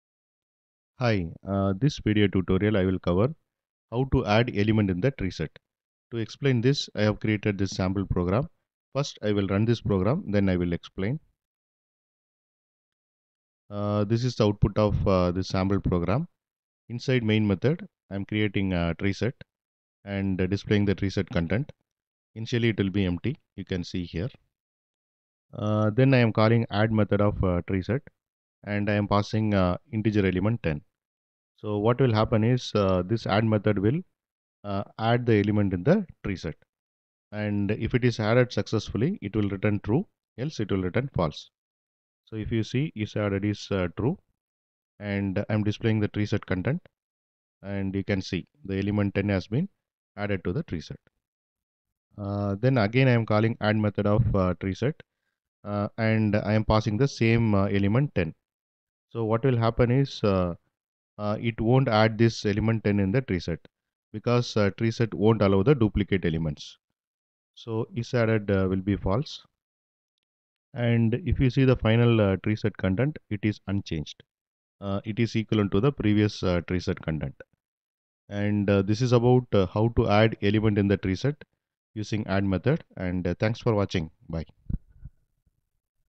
How to Add Objects to TreeSet | TreeSet (Add Object)
Welcome to our Java Collections tutorial series! In this video, we'll explore how to add objects to a TreeSet in Java. TreeSet is a powerful data structure that implements the Set interface and maintains elements in sorted order, making it an excellent choice for scenarios where you need a sorted collection of unique elements.

- Introduction to the TreeSet class and its features
- Step-by-step guide on adding objects to a TreeSet
- Understanding the requirements for objects to be added to a TreeSet (implementing Comparable interface or providing a Comparator)
- Practical examples and coding demonstrations to illustrate the process
- Best practices for adding custom objects to a TreeSet
- Tips on maintaining the natural ordering or defining custom ordering for objects in a TreeSet

**Why This is Important:**
Adding objects to a TreeSet is a fundamental operation when working with sorted collections in Java. Understanding how to properly add objects ensures that your TreeSet maintains its sorted order and uniqueness invariant, which is crucial for many applications.

How to add Object to the TreeSet? | TreeSet (Add Object)